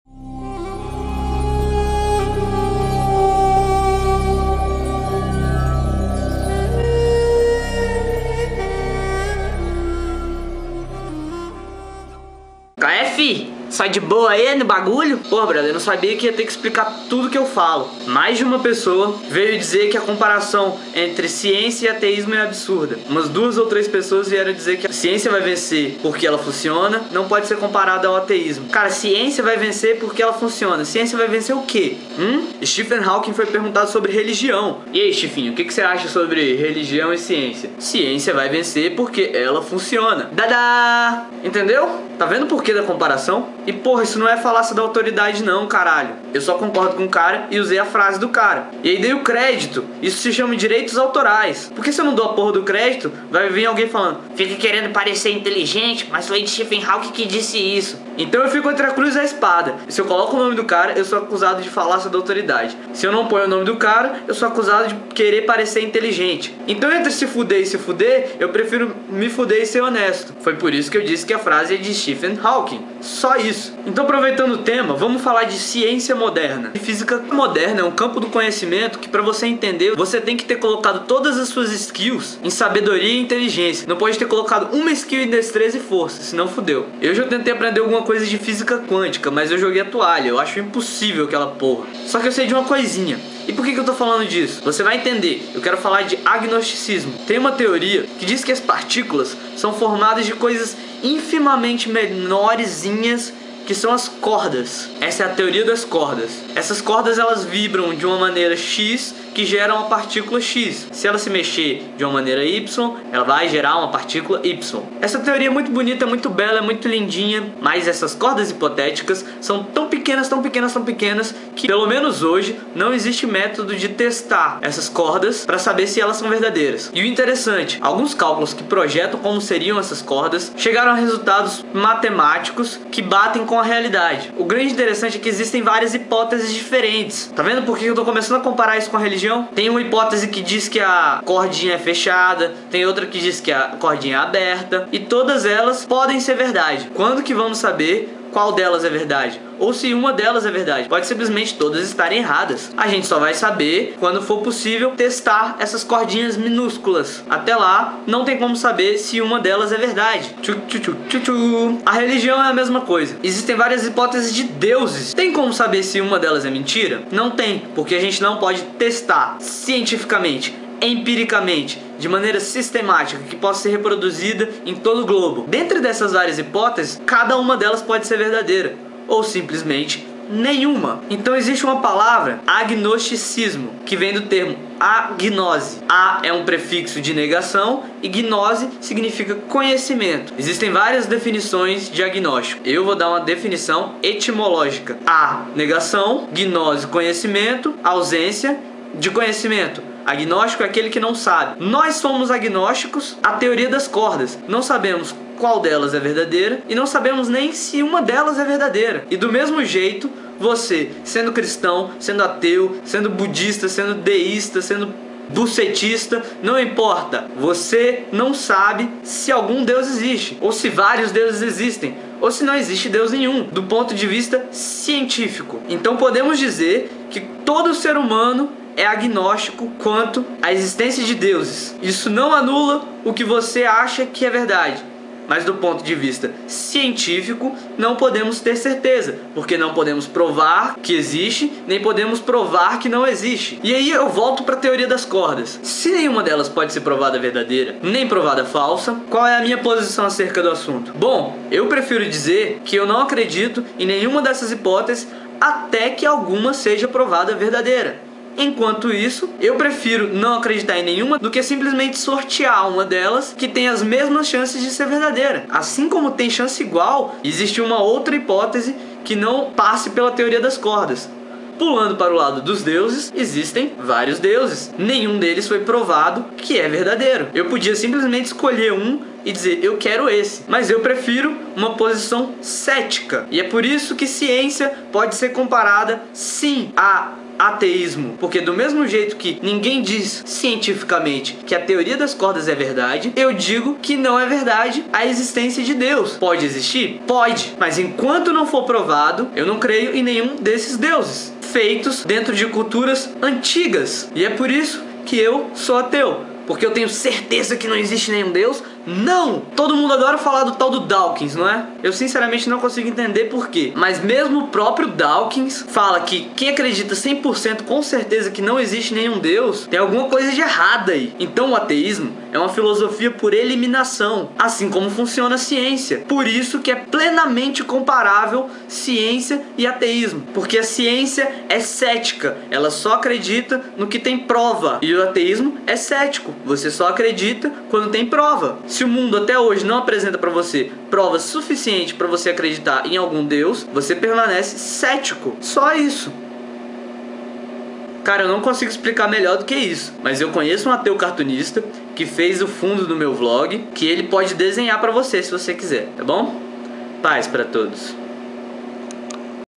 Ateu Informa: Física Moderna & Ateísmo Científico
Deixa eu sanar qualquer dúvida que tenha restado:
Teoria das Cordas = Uma teoria racional para explicar como funcionam as partículas. Não pode ser considerada verdadeira ainda pois não podemos prová-la.
Teoria do Boi-Feio-Azul = Uma teoria maluca que inventei agora pra dizer como funcionam as partículas.
Religião = Teoria do Boi-Feio-Azul